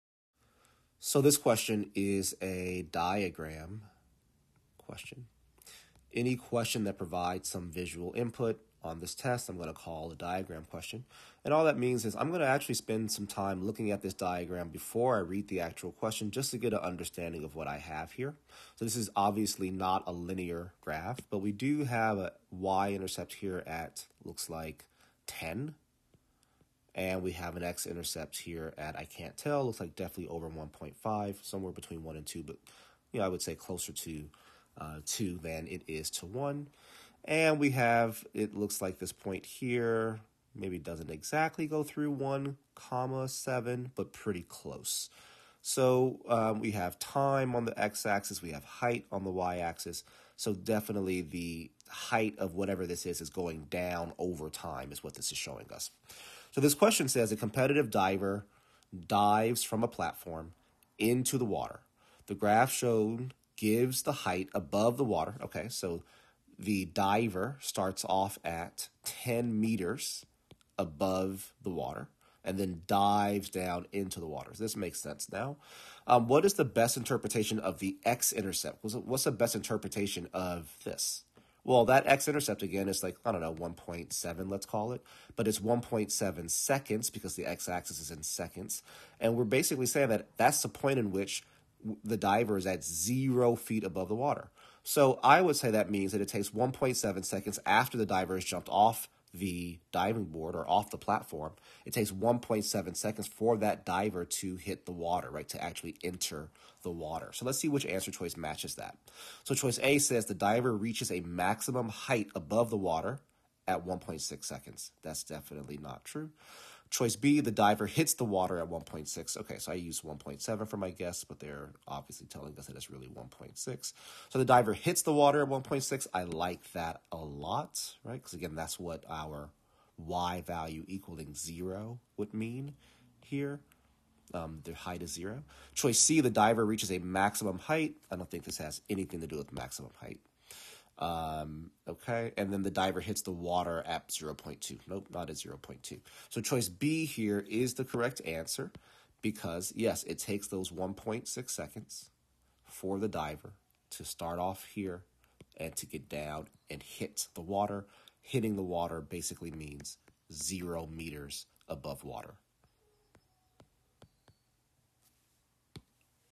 A competitive diver dives from a platform into the water. The graph shown gives the height above....
Nonadaptive Digital SAT Practice Test 5, Section 2, Module 2, Question 9:

A competitive diver dives from a platform into the water. The graph shown gives the height above the water y, in meters, of the diver x seconds after diving from the platform. What is the best interpretation of the x-intercept of the graph?